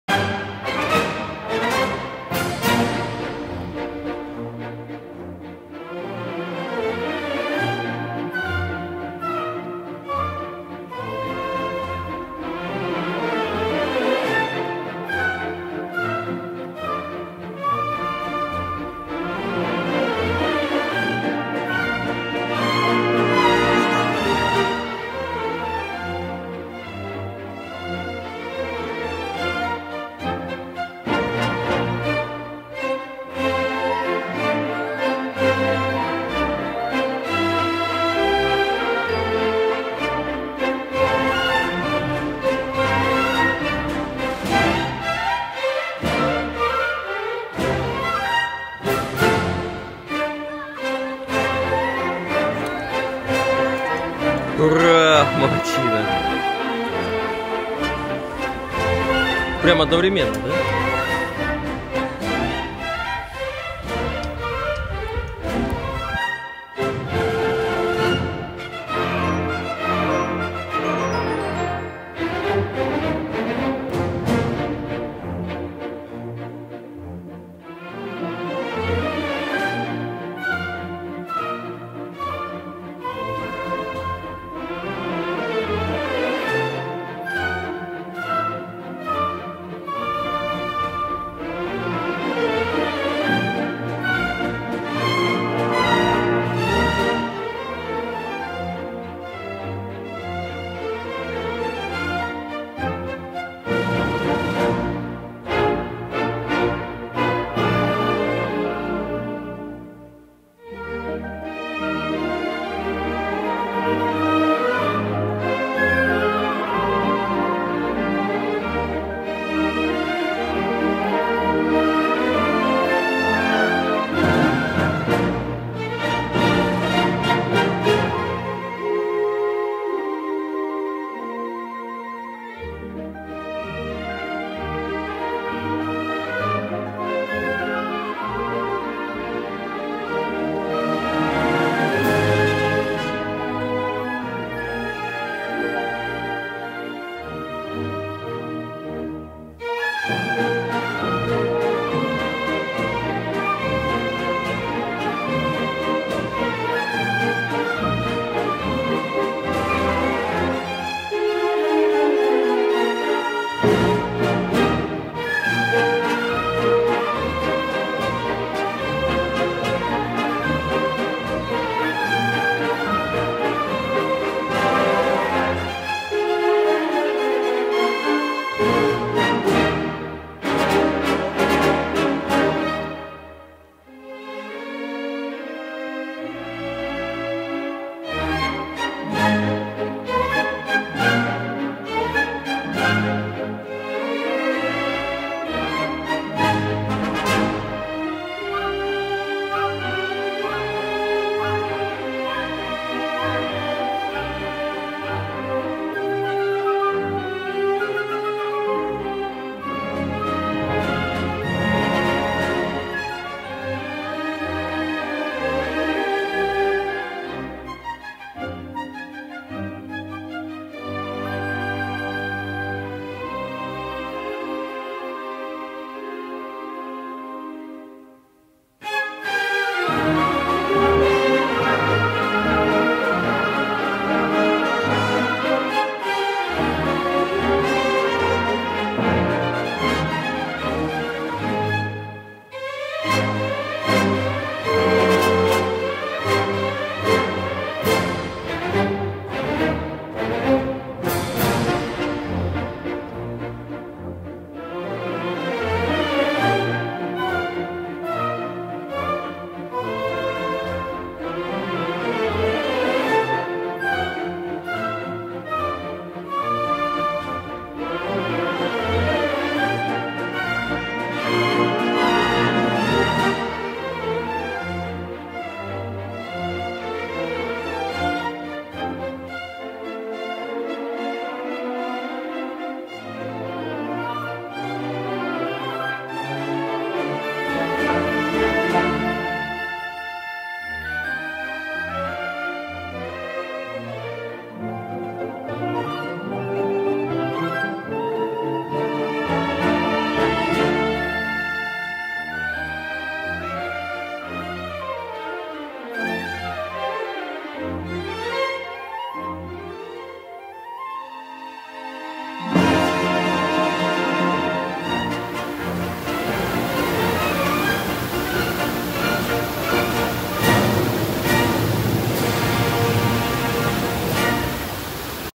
210-2 Рыбалка, Клёвое место, Савельево 1082
Рыбалка, Клёвое место, Савельево, Ленинградское шоссе (М-10), от МКАД = 37 км, повернуть в сторону СНТ Пешки, ехать до Рыболовный пруд Савельево 2,5 км (Клевое место).